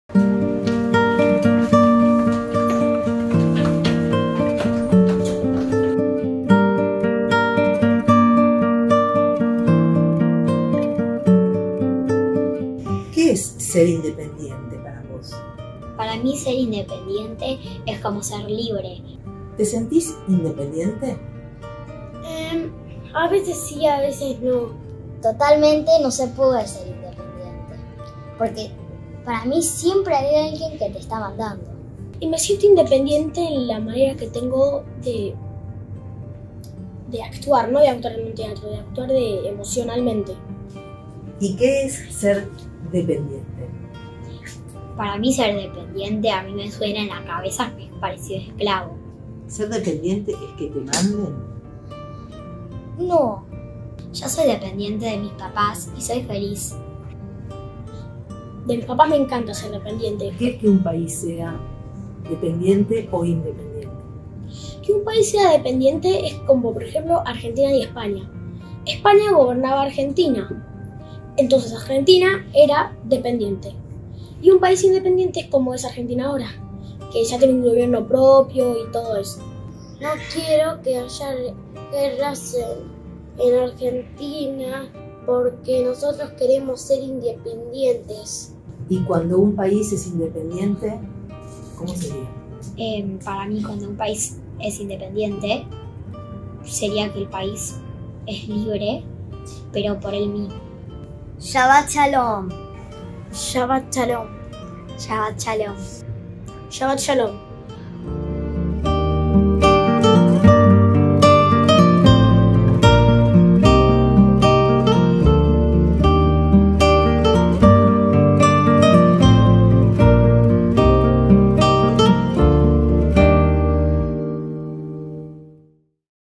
SHABAT SHALOM (Edición Nº 94) 8 de Julio 2022
Invitamos a conectarse a las redes de la escuela, hoy viernes 8 de julio 2022, para compartir junto a nuestra escuela un momento antes del Shabat.